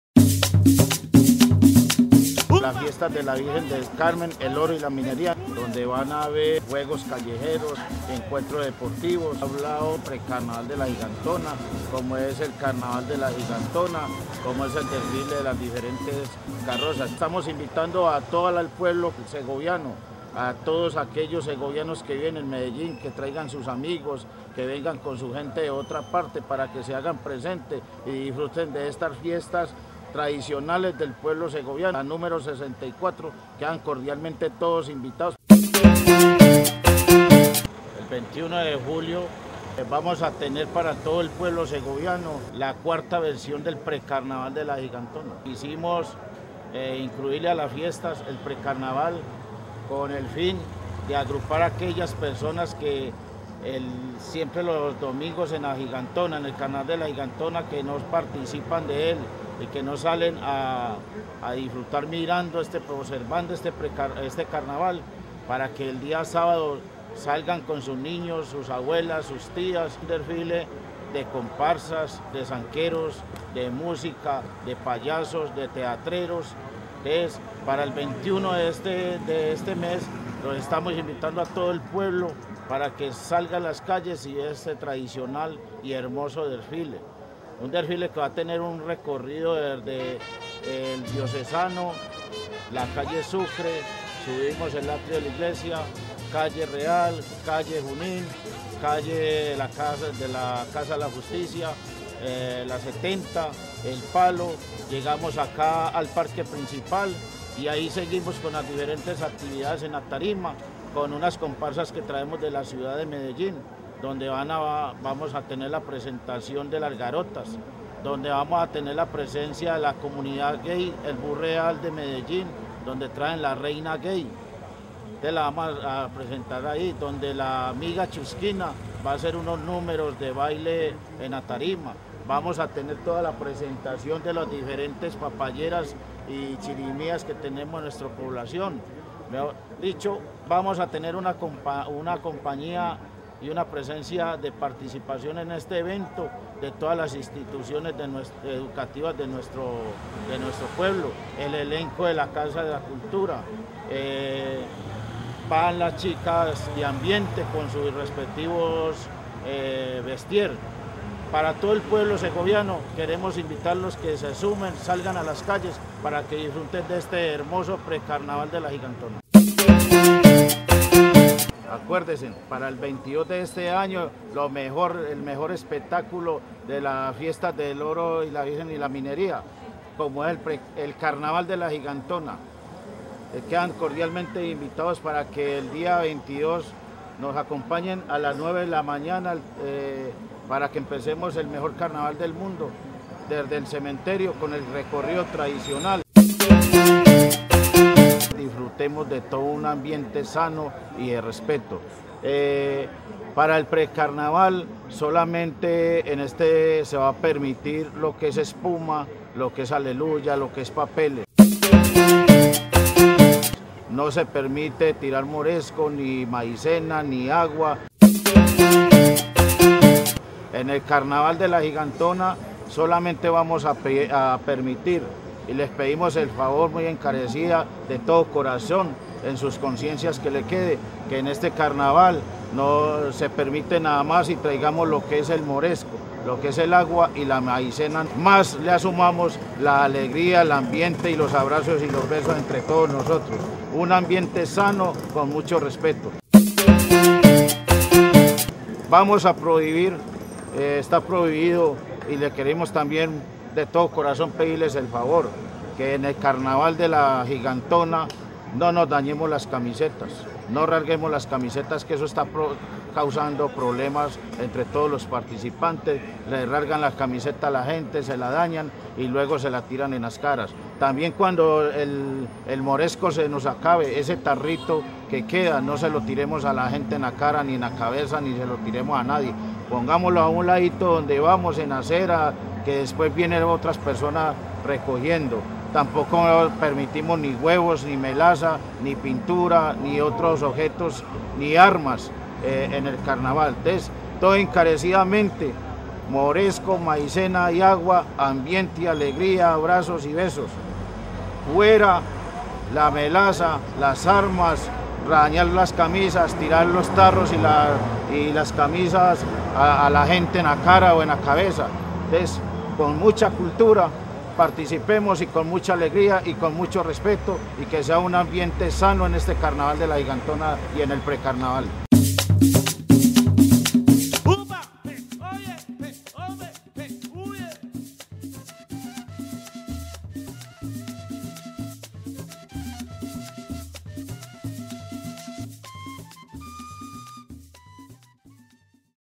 INVITACIÓN CARNAVAL DE LA GIGANTONA 2012
carnaval de la gigantona en las 64 fiestas de la virgen del carmen, el oro y la minería, en Segovia 16 al 22 de julio de 2012